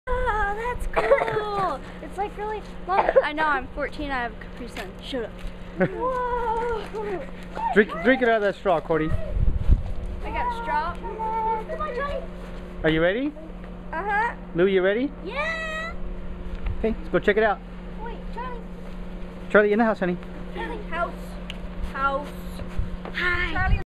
Linkin Park and Prodigy concert, VIP limo service 2-19-2011
I'm taking the kids to the Linkin Park and Prodigy concert held at the MGM Grand Garden arena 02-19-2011. The kids were in complete amazement. This is their first limo ride. The concer was absolutely amazing. We had VIP passes, early entry, food, drinks, swag, (freebies), early shopping and front rail center viewing. The band handed the kids guitar picks which made their night.